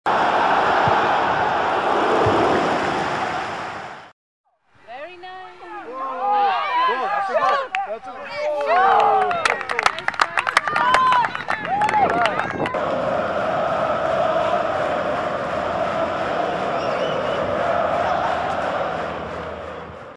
TOCATV: U13 Boys HOYAS (Shot by Carlos)
MSI Classic
Saturday, April 16 - 1:50 PM
U13 Boys HOYAS vs Blue Dragons
Soccerplex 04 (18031 Central Park Circle, Boyds, MD 20841)

#‎20thanniversary #‎WeAreToca #‎TOCA #‎PLAYsimple #‎soccer #‎MSIClassic